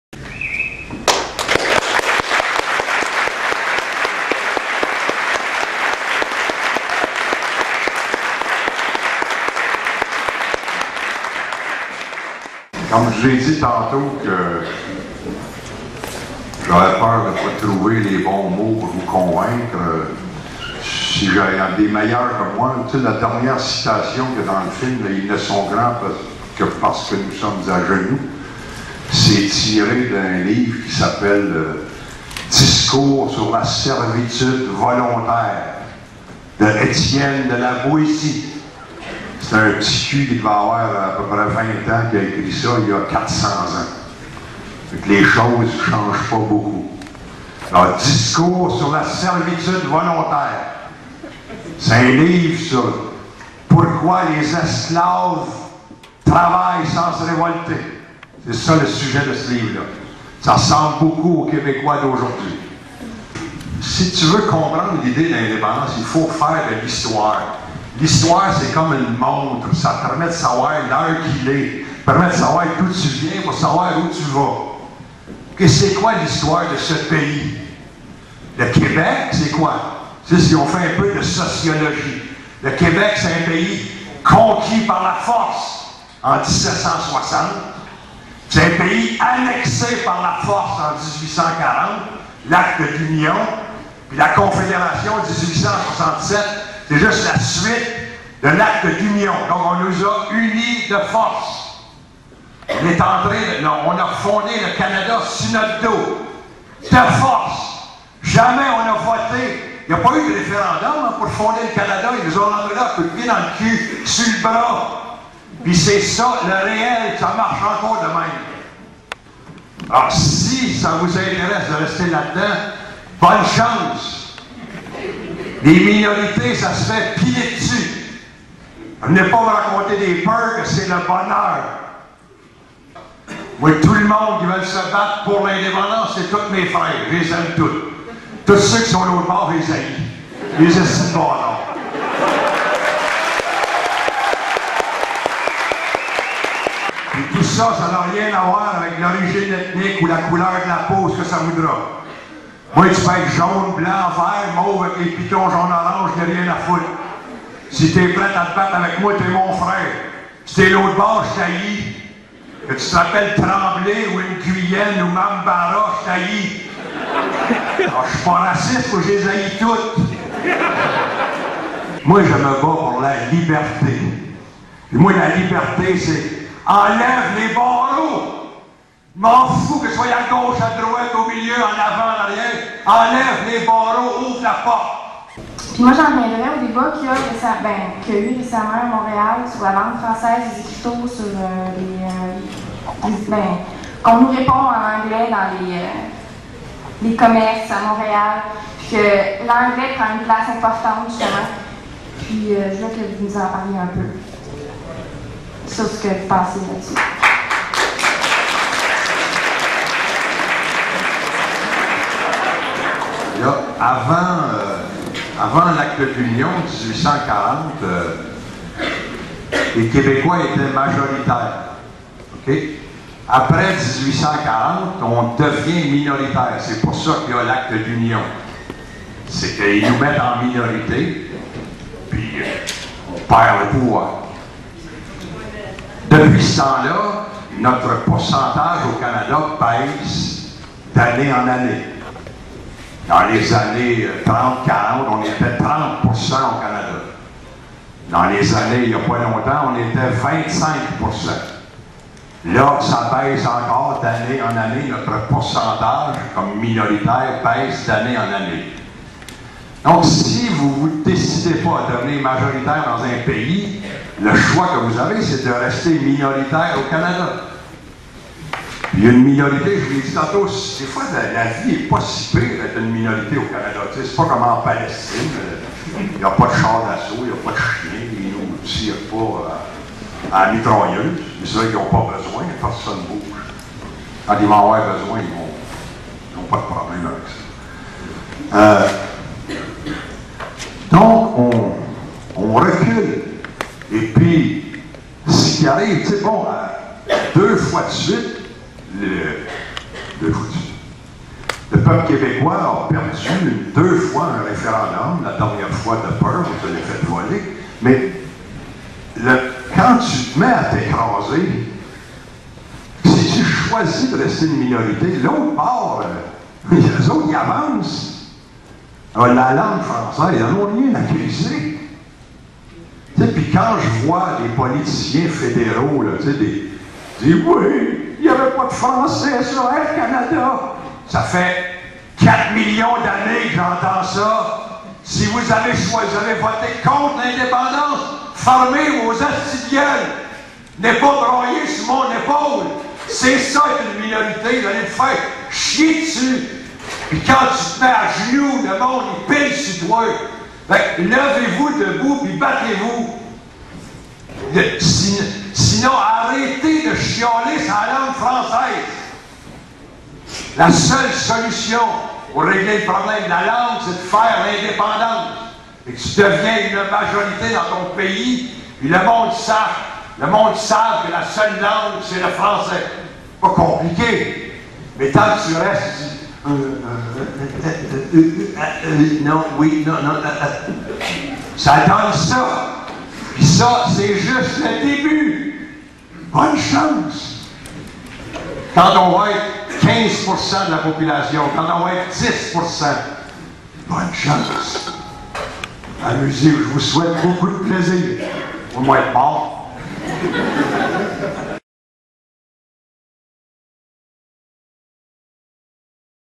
Falardeau's best speech
This propaganda page dedicated to Québec's freedom fight couldn't be complete without a speech by Pierre Falardeau. Here, Falardeau runs the show, and a good one too. This is a truly Patriotic speech, in the great tradition of early American Republicanism, full of cynicism, wit, historical insight and humor. Again, Falardeau reveals to be a master of sarcasm, and his sarcasm is broad-ranged, aimed altogether at enemies of Québec and at bleeding-hearted defenders of Québec's "autonomy" and "special status" within the canadian Union. No one is left unscorched by Falardeau, whose arguments ram into clichés and half-witted commonly-received ideas like a runaway freight.

This speech was given in early 2008 to underline the launch of the Sherbrooke, QC chapter of the Québécois Resistance Network (Réseau de Résistance du Québécois, a/k/a R.R.Q.), in front of a sold-out audience whose average age ranged around 25. It was originally posted on Youtube through resistancequebecoise's channel.